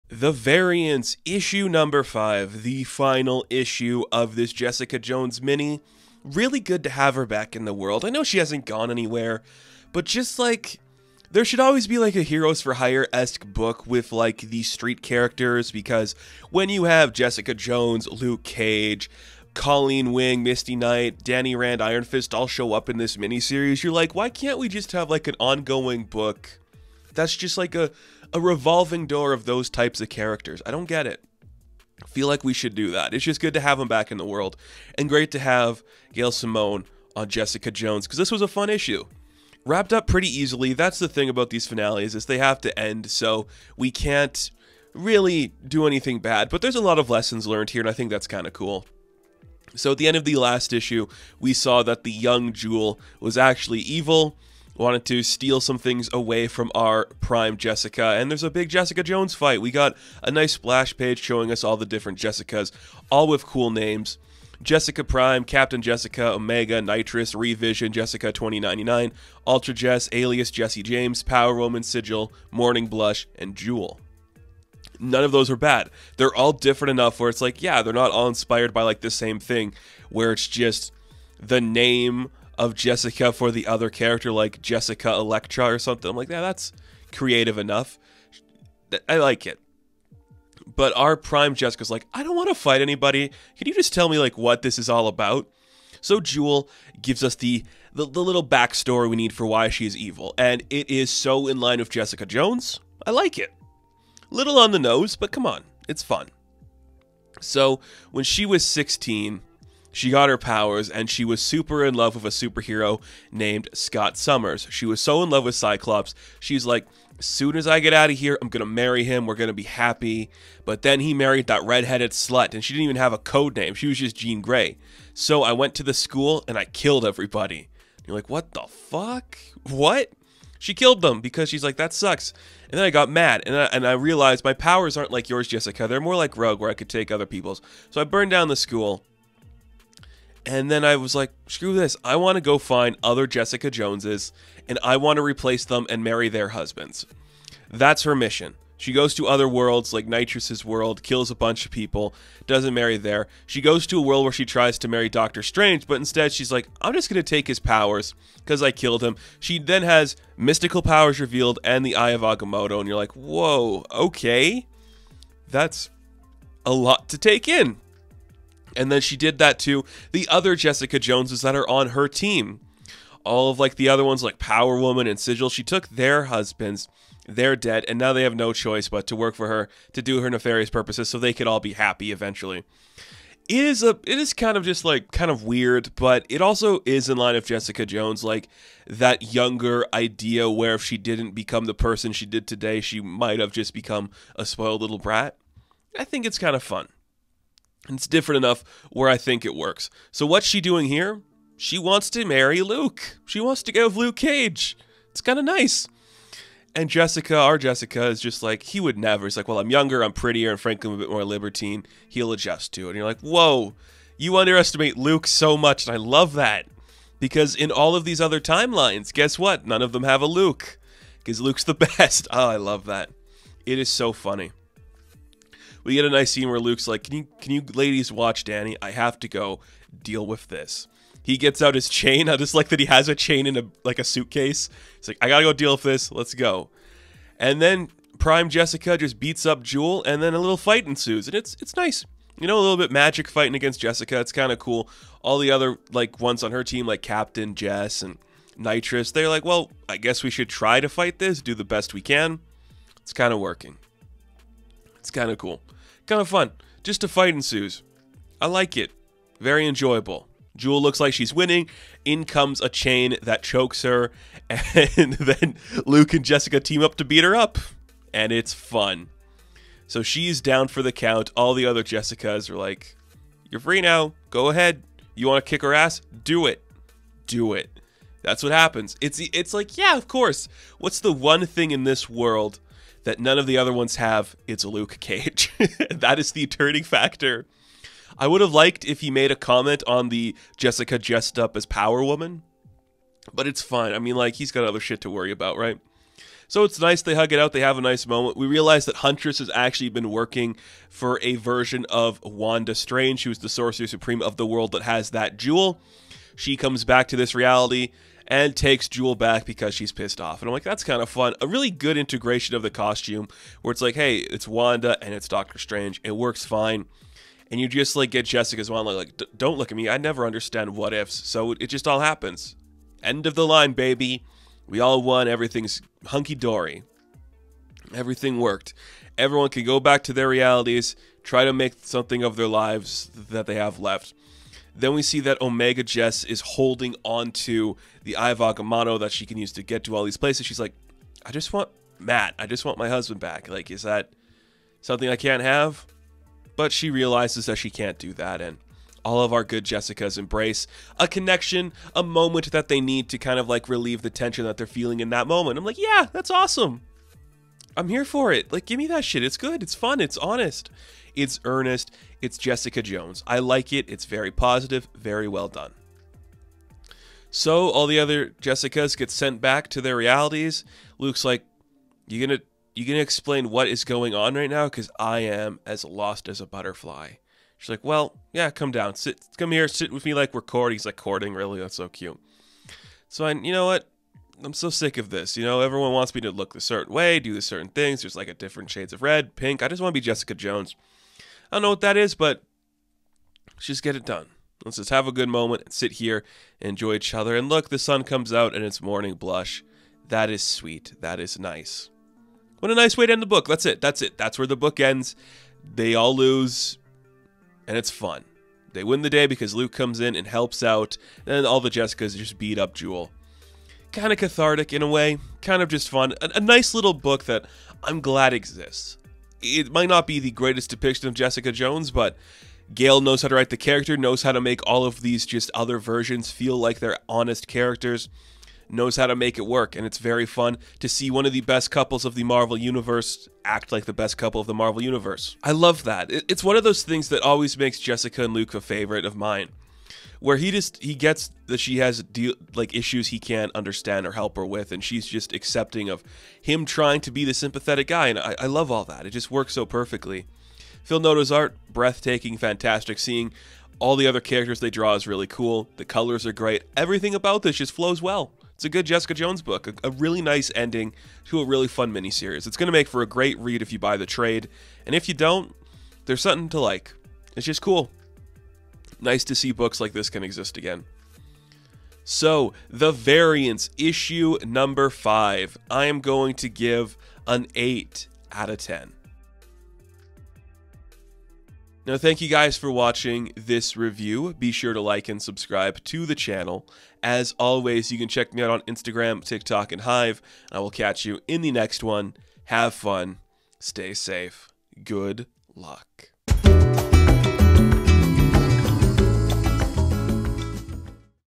Jessica Jones In... The Variants #5 (Finale): What Ever Happened to Baby Jess?
The final issue of Jessica Jones Vs. herself is upon us. Witness the origin of Evil Jess, and why going boy crazy is never the right thing to do. Plus, let's all just love Luke Cage a little harder tonight.